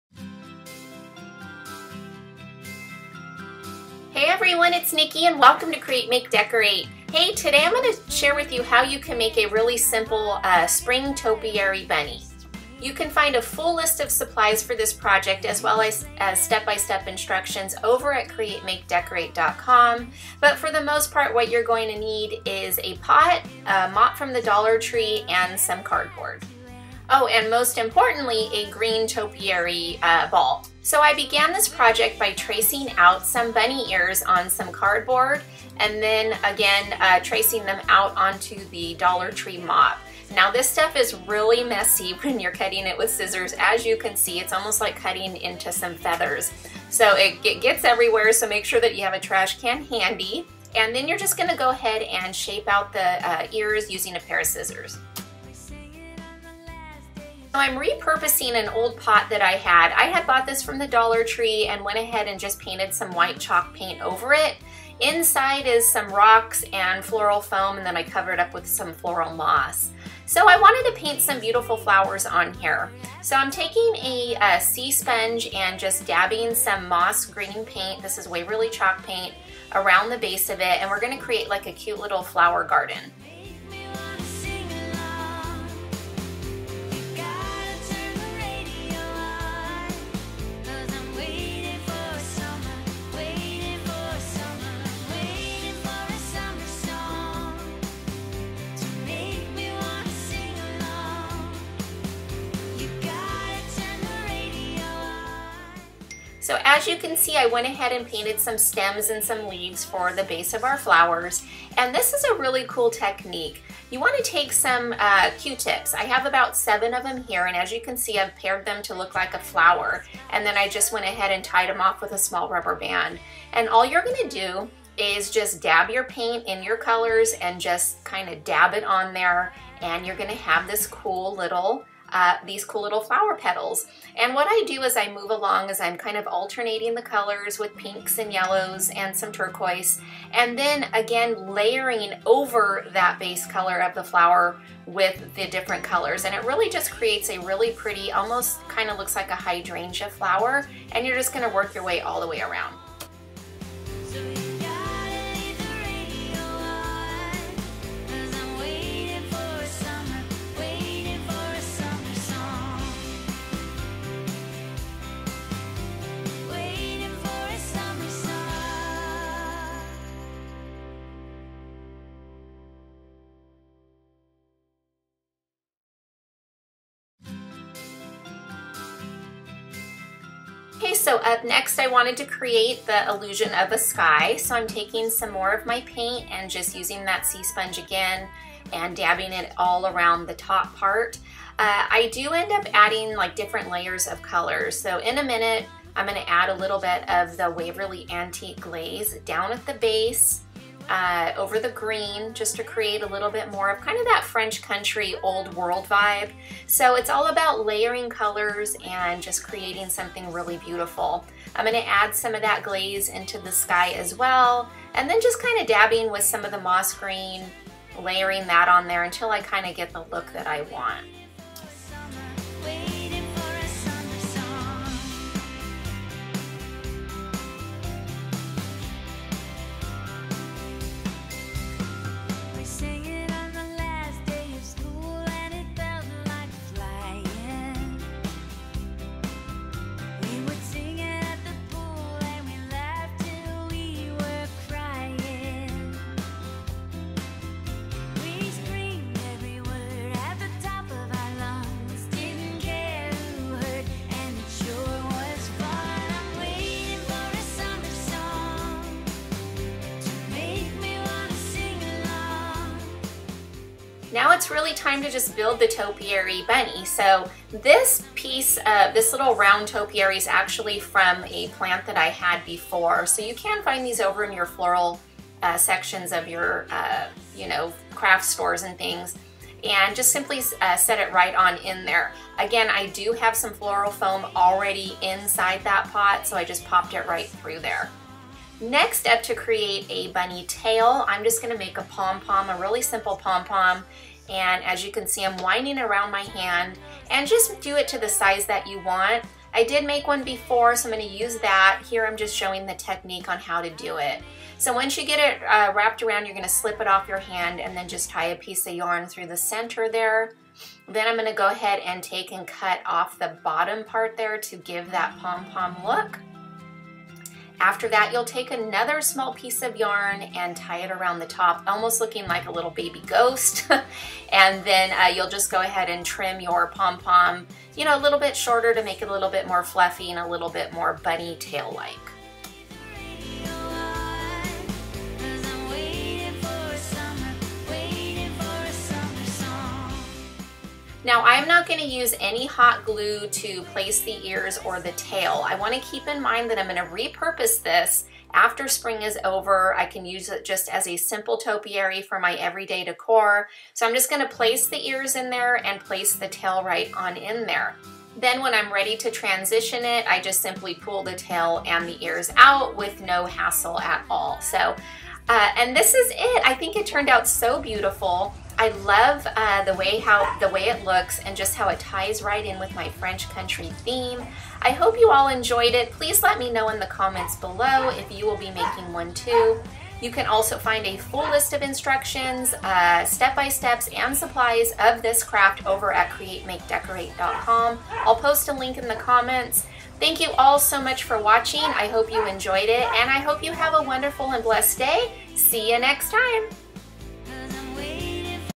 Spring Bunny Topiary
How to make a Spring Bunny Topiary that is easily transitional to everyday decor!  You can find a full list of supplies and a step by step tutorial